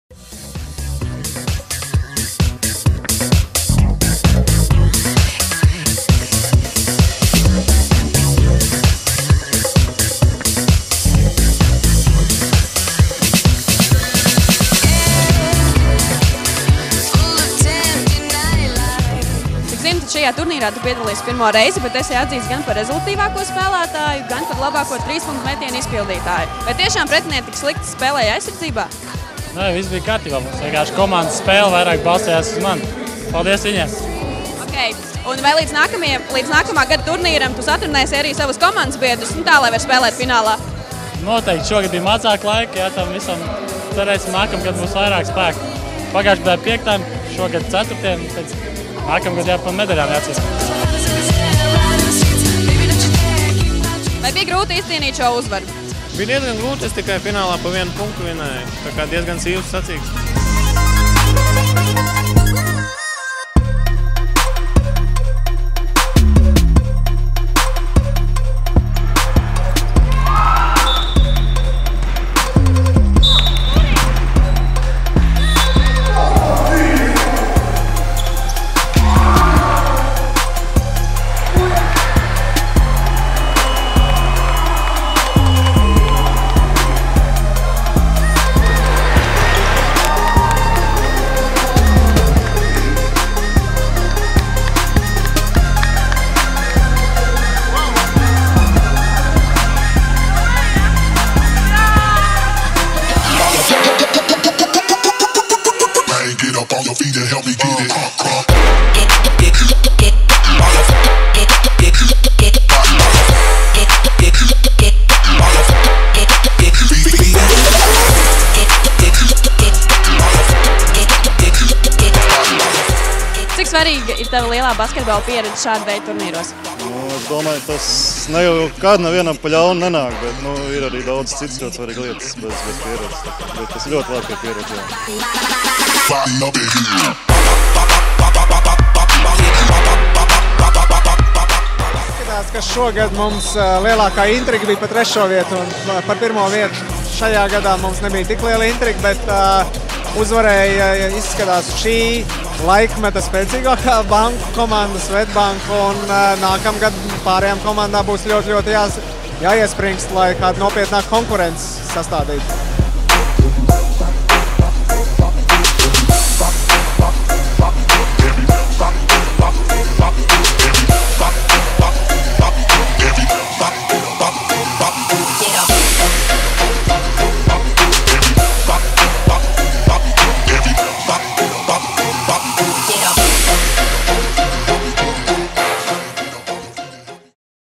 Latvijas Banku basketbola kauss 2013. Fināldiena
LBBK 20. turnīrā vīriešu konkurencē uzvarēja Swedbank, apsteidzot Hipotēku banku un SEB banku. Turnīra vērtīgākais spēlētājs - Andis Pogiņš (Swedbank), rezultatīvākais spēlētājs - Kārlis Ozoliņš (Citadele banka), trīspunktnieku konkursa uzvarētājs - Mareks Vonsovičs (Hipotēku banka). Sieviešu 3:3 turnīrā triumfēja Citadele banka, finālā apspēlējot DNB banku. Eksperta lomā - Latvijas Komercbanku asociācijas prezidents Mārtiņš Bičevskis